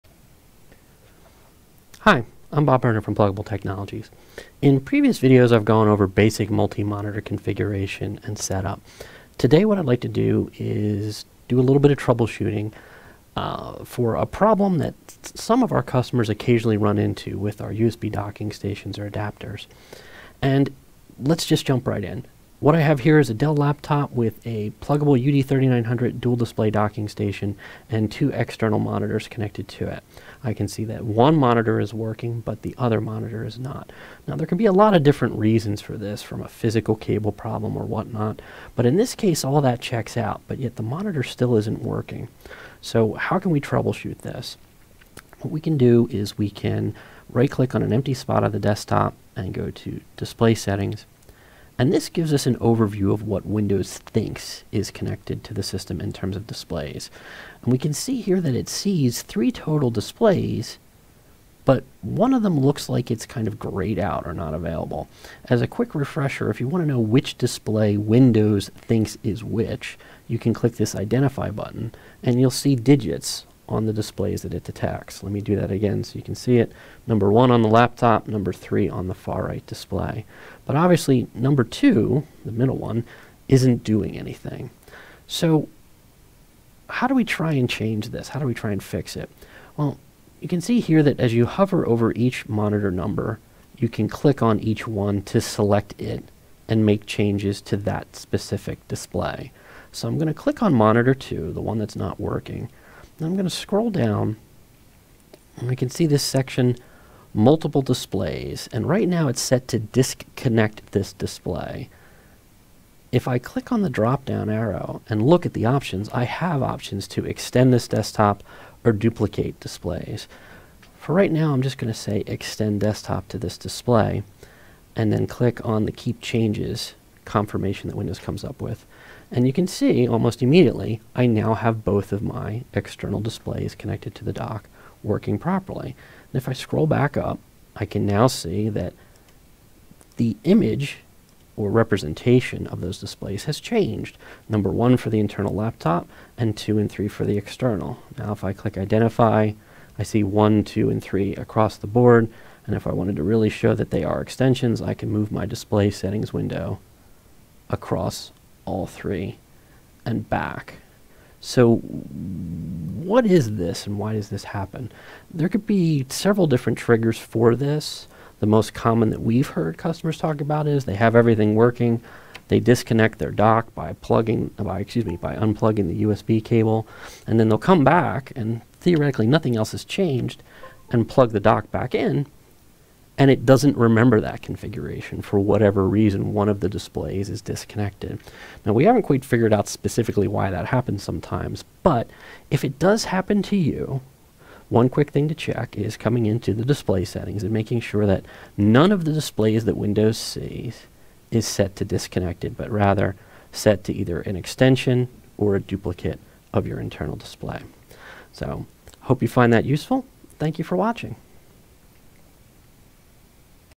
Troubleshooting A Disconnected Display with the UD-3900
In this video I go over one method to troubleshoot a disconnected display using our UD-3900 Dual Display Docking Station and Windows 10.